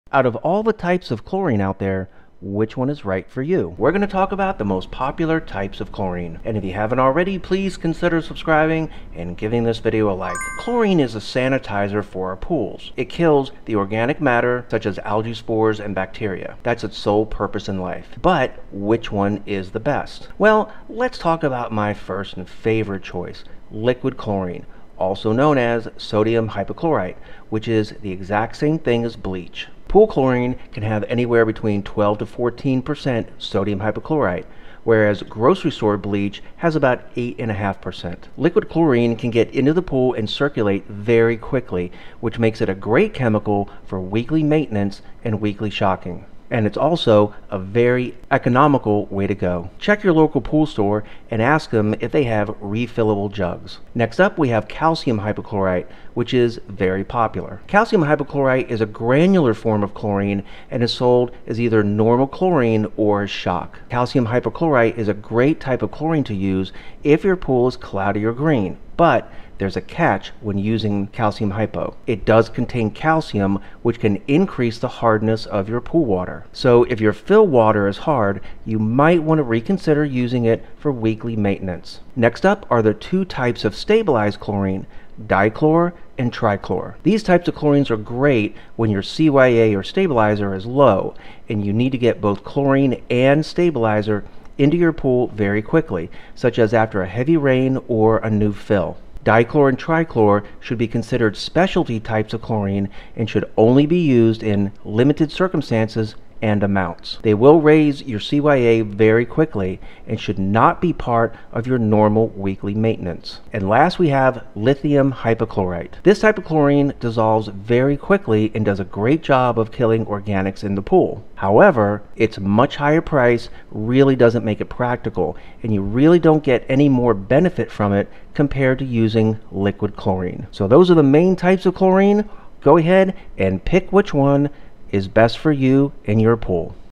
What Pool Chlorine Is Best? | Get To Know Your Chlorine.
What Type Of Chlorine Is Best?

Chlorine is the sanitizer for your pool.  It kills the organic matter such as algae spores and bacteria.  That's its sole purpose in life.  But what's the best kind of chlorine?

My first and favorite choice is liquid chlorine, also known as sodium hypochlorite, which is the same thing as bleach.  Pool chlorine can have anywhere between 12 - 14% sodium hypo, whereas grocery store bleach has about 8.5%.  It gets into the pool and circulates very quickly which makes it a great chemical for weekly maintenance and weekly shocking.
And it's also a very economical way to go.  Check your local pool store and ask if they have refillable jugs.

Next we have calcium hypochlorite, which is very popular.  It's a granular form of chlorine and is sold either as normal chlorine, or as shock.  Cal hypo is a great type of chlorine to use if your pool is green or cloudy, but there's a catch when using calcium hypo.  It does contain calcium which can increase the hardness of your pool water.  So if your fill water is hard, you might want to reconsider using it for weekly maintenance.

Next up is the two types of stabilized chlorine, dichlor and trichlor.  These types of chlorine are great when your CYA/stabilizer is low and you need to get both chlorine and cya into your pool, such as heavy rains or a new fill.  Dichlori and trichlor should be considered special types chlorine and should only be used in limited circumstances and amounts.  They will raise your CYA quickly and should not be used as part of your normal weekly maintenance.

And last we have lithium hypochloriite.  This type of chlorine dissolves quickly and does a great job of killing organics in the pool.  However, it's much higher price really doesn't make it practical, and you really don't get any more benefit from using it, compared to liquid chlorine.